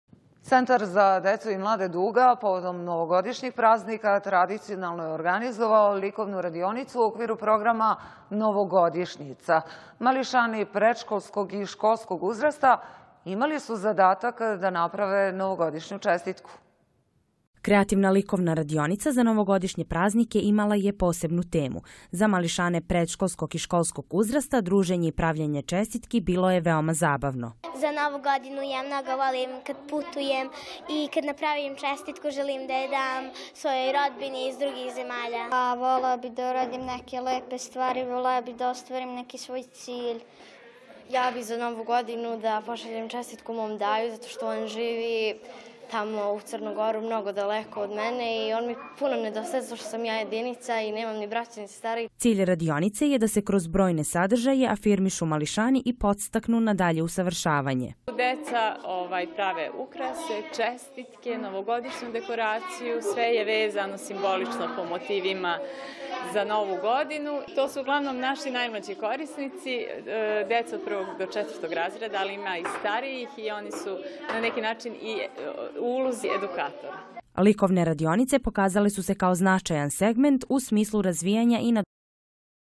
Likovna radionica povodom Nove godine
Centar za decu i mlade “Duga” povodom novogodišnjih praznika tradicionalno je organizovao likovnu radionicu u okviru programa "Novogodišnjica". Mališani predškolskog i školskog uzrasta imali su zadatak da naprave novogodišnju čestitku.